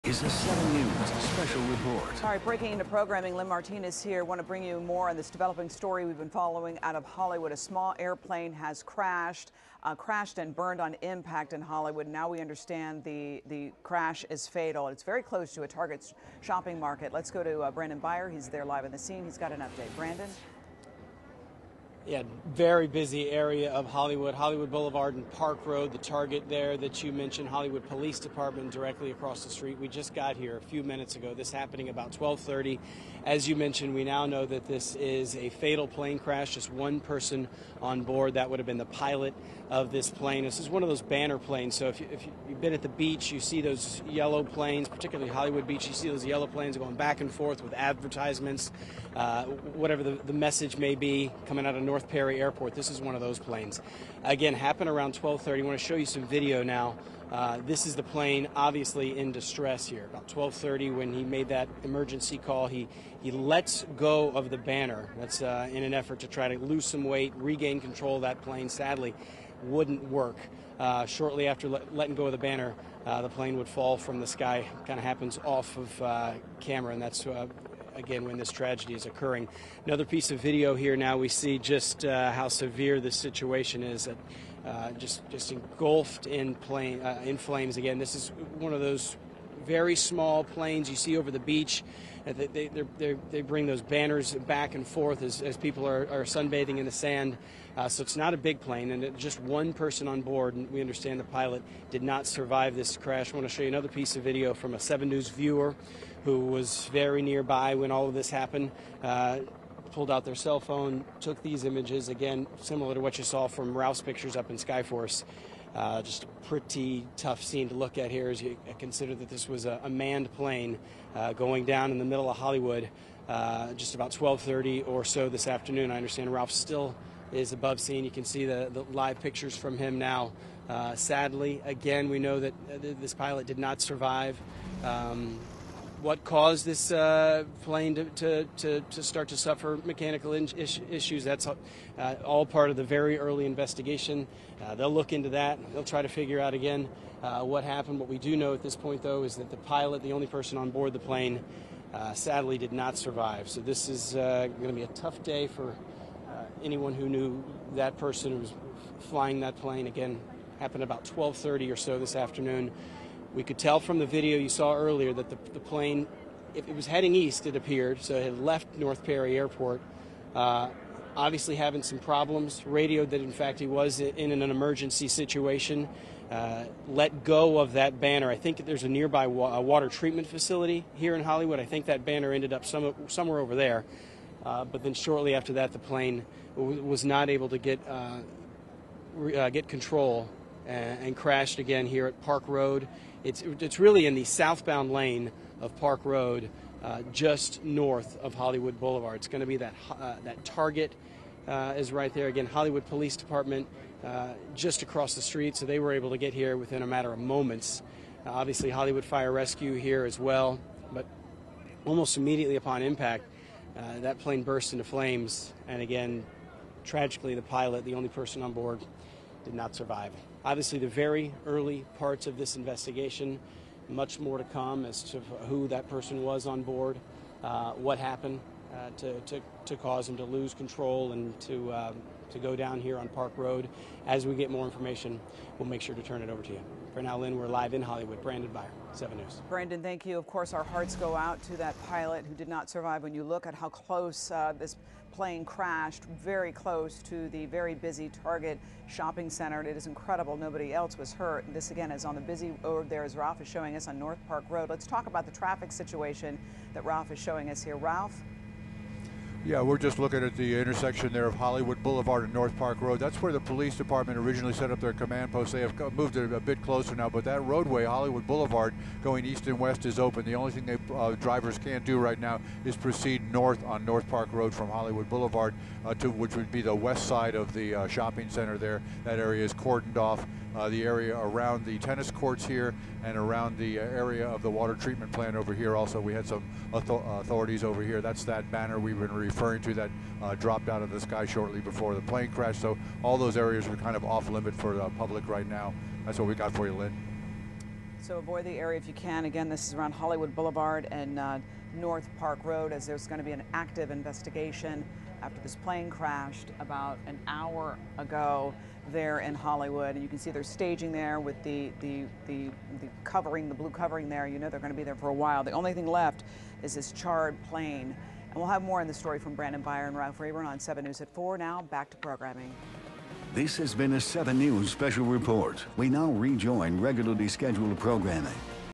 Banner plane crashes and engulfs in flames near Memorial Regional Hospital in Hollywood; 1 dead
One person has died after a banner plane crashed on a road adjacent to Memorial Regional Hospital in Hollywood and burst into flames.